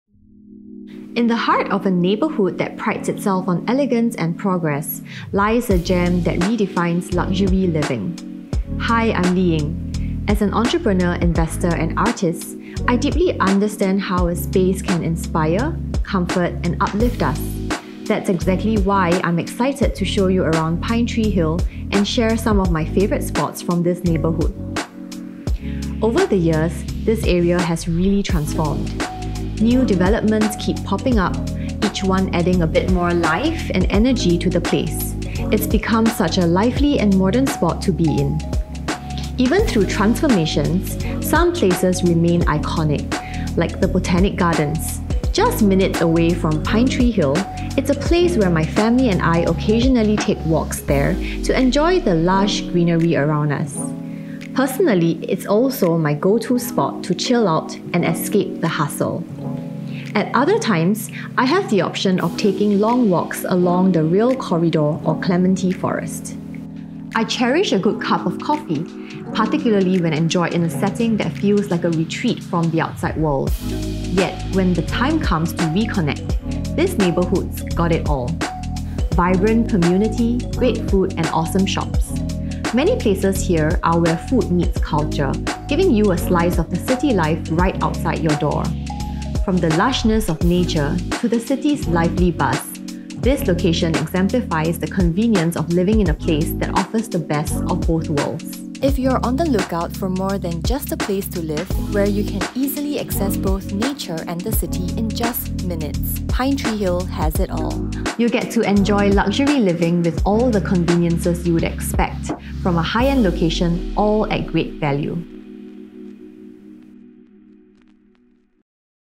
Pinetree Hill by UOL (feat. Liying) - Modern Living In The New Era
With its proximity to vast greenery, lifestyle enclaves, and renowned schools, the new Pinetree Hill development is set to be one of Singapore’s most desirable addresses.

Read more on Portfolio: